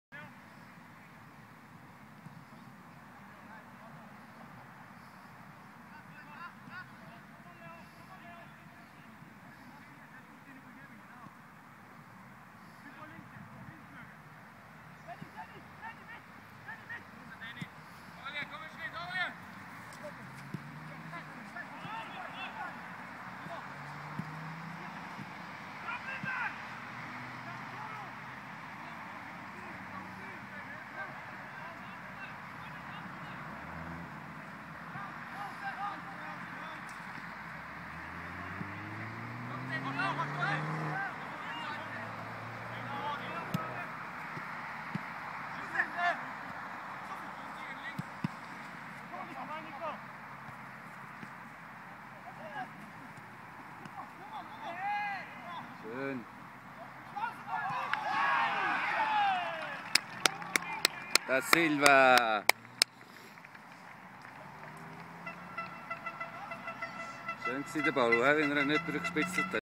SC YF Juventus - BSC Old Boys_2 - 0_Mychell Da Silva_17.11.2012.MOV
Sehr schön gespitzelt von Balu Ndoy auf Almir Murati der schön weiterleitete auf Mychell Da Silva der verwandelte.